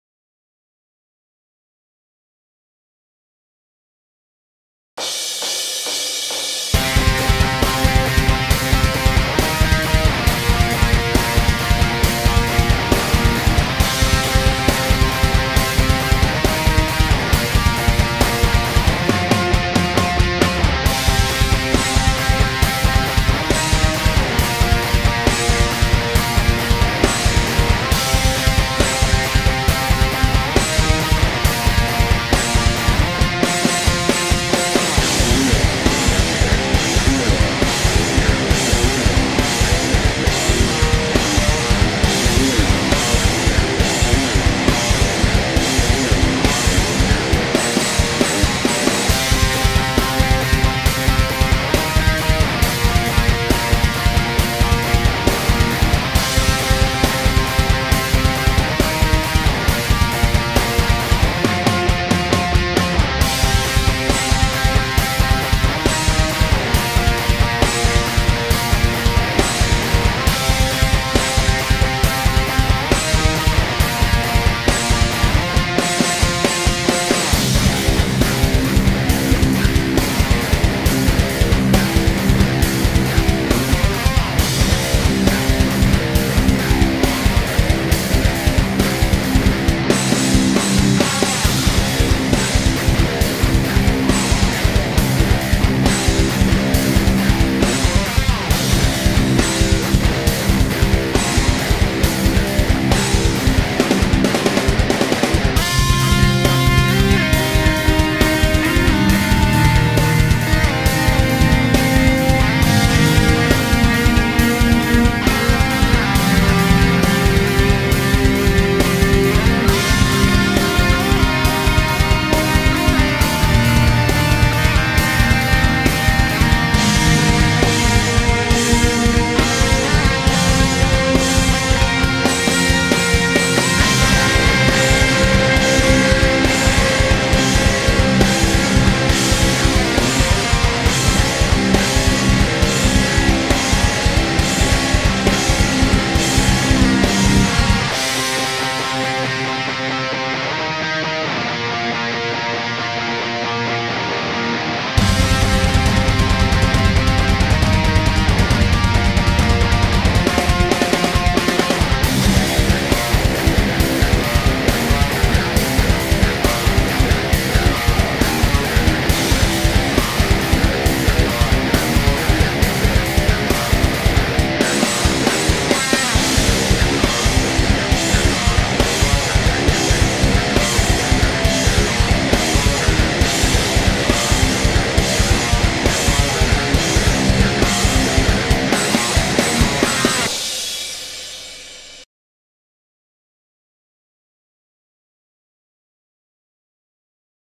Djent Instrumental 2015 - Blackstar ID Core
Music composed arranged and recorded by me thru FL Studio using EQ plugins and EZ Drummer.

Amp used is a Blackstar ID Core using Blackstar's Insider.